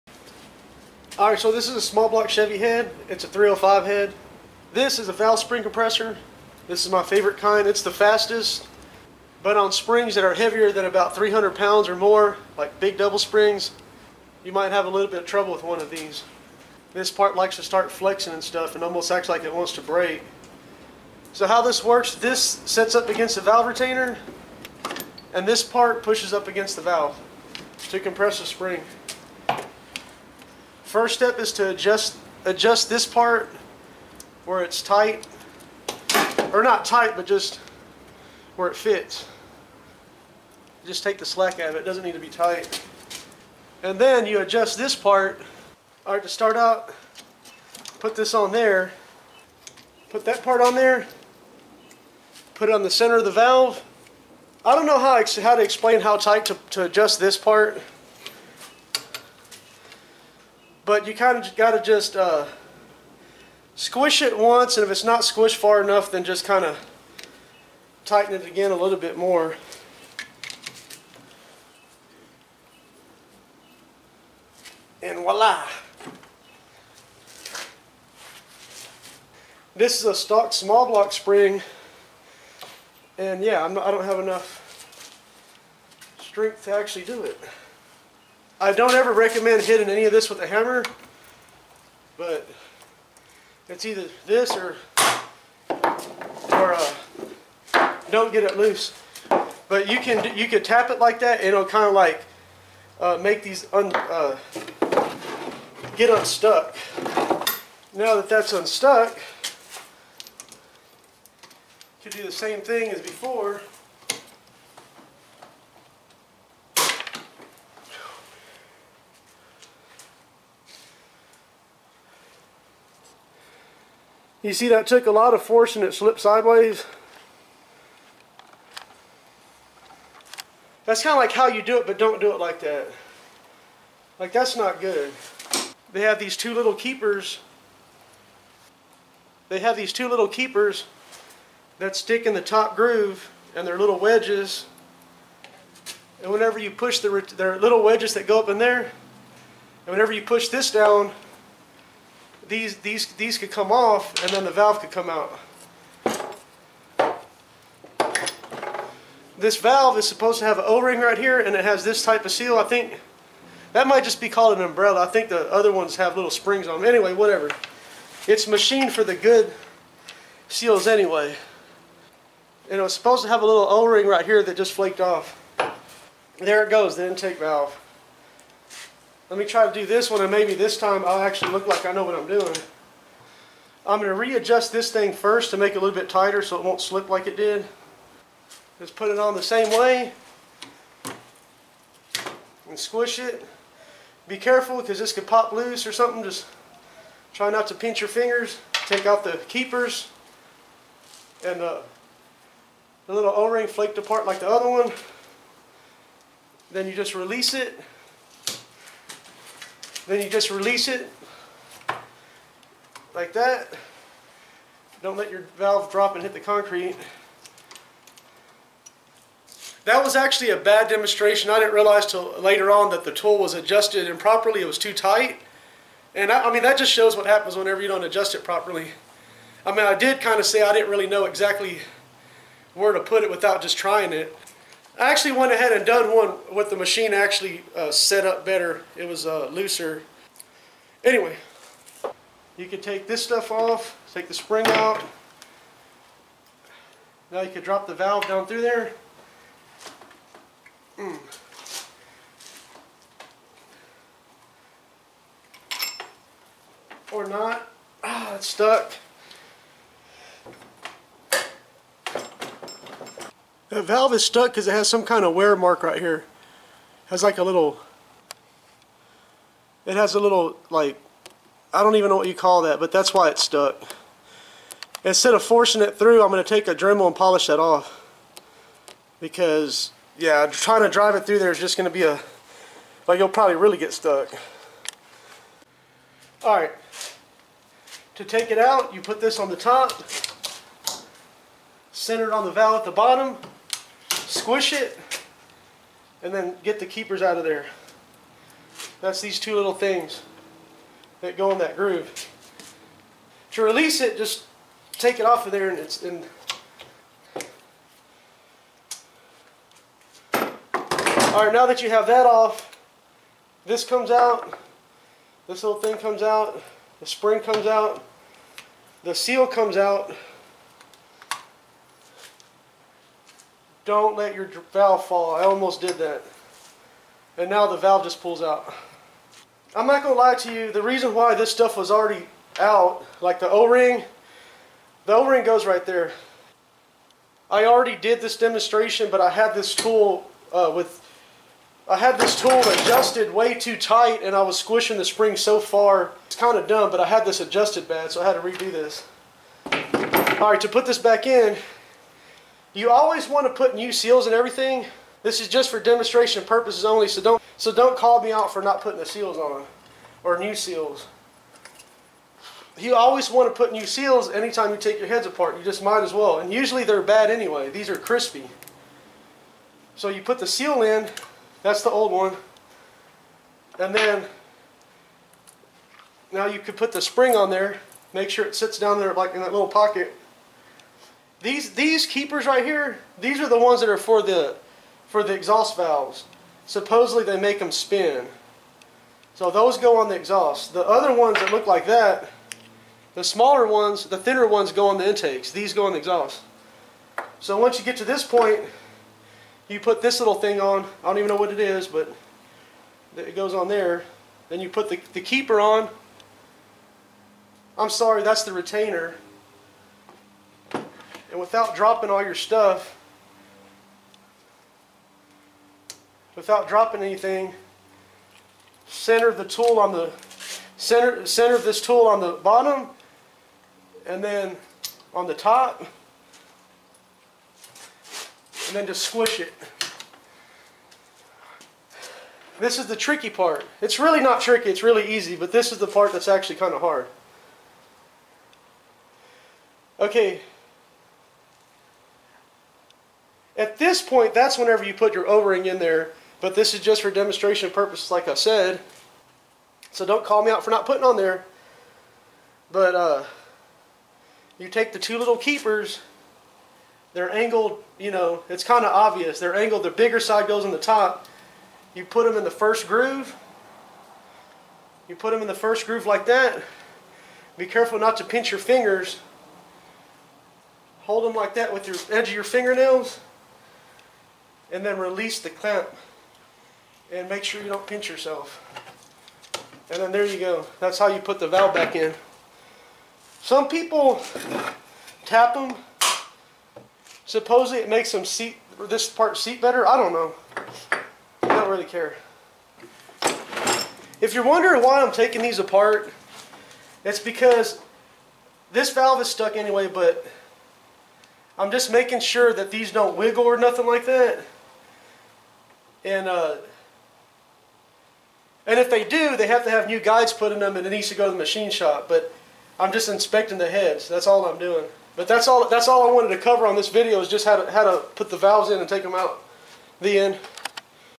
How to remove valves on small block chevy heads
In this video I show how to remove and install valves on a small block chevy head. I use a valve spring compressor.  These heads are factory 305 sbc heads with stock springs.  I was taken the valves out to check the valve guides.  Just getting stuff ready for my 305 small block chevy build.